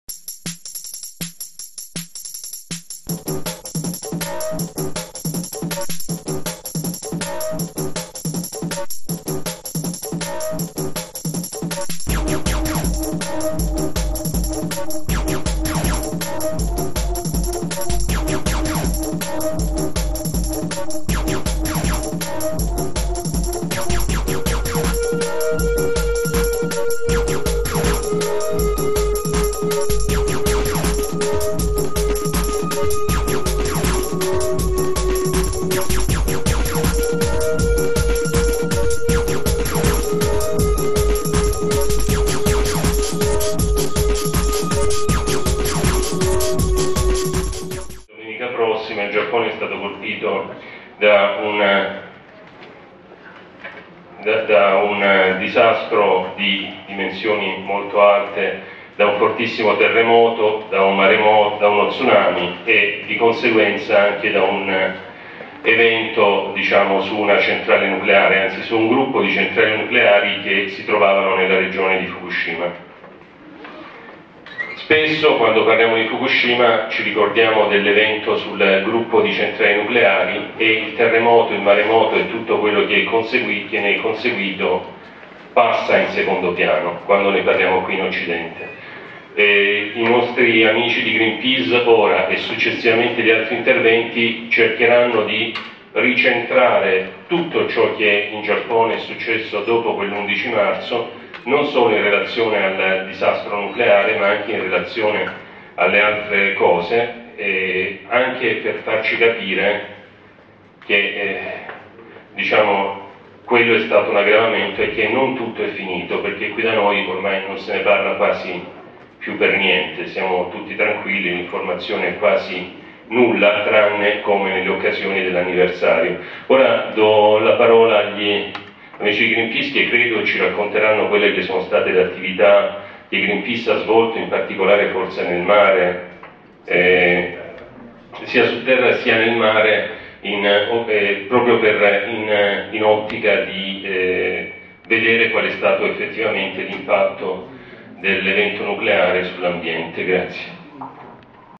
A un anno da Fukushima - Introduzione
Università di Pisa - Polo Fibonacci. Conferenza sui pericoli del nucleare.